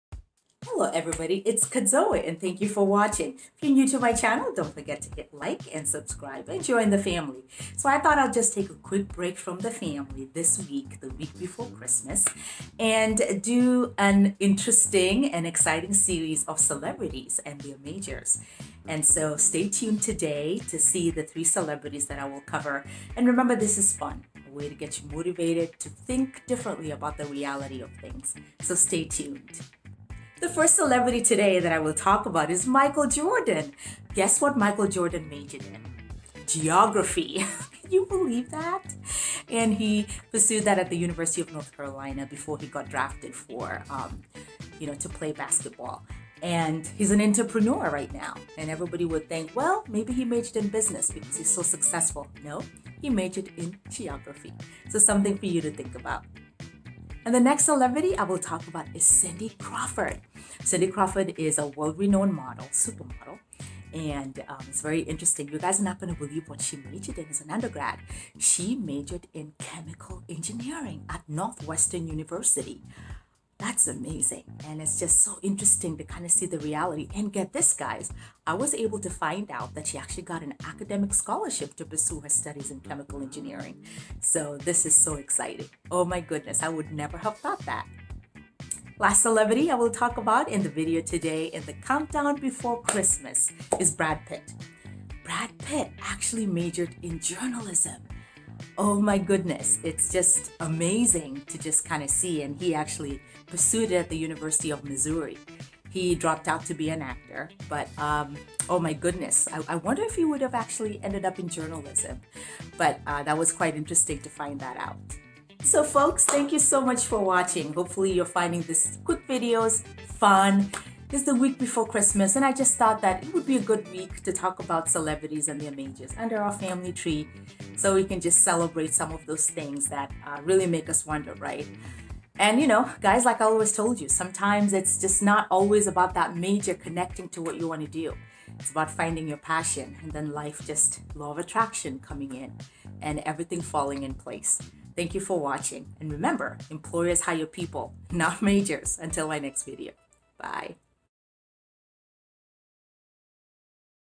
CELEBRITIES & THEIR MAJORS: BRAD PITT CINDYCRAWFORD & MICHAEL JORDAN
Kadzowe is a Career Expert, Empowerment Specialist and Youthologist for Kadzoweforafrica.com She has over 10 years of experience coaching people on how to strategize for success.
Michael Jordan
College: University of North Carolina
Major: Geography
Dropped out his junior year.
Cindy Crawford
College: Northwestern University
Major: Chemical engineering
Received an academic scholarship but only spent one quarter here before dropping out to pursue modeling.
Brad Pitt
College: University of Missouri
Major: Journalism
Dropped out two weeks before he graduated.